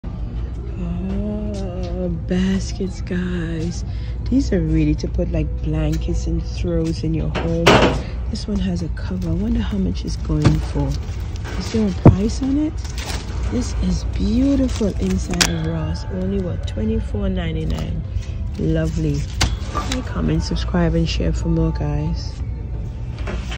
Ross Finds - Creative Basket Ideas 🧺
Ross Dress for Less has all those Baskets to use for your home. There are soo many creative uses for baskets these days you cant go wrong with having a few in your home. Come browse with me at Ross Dress For Less and maybe we can find the next great Ross Find.

Please don’t forget to put God FIRST 🏆 in everything you do.

I want to thank everyone for your support.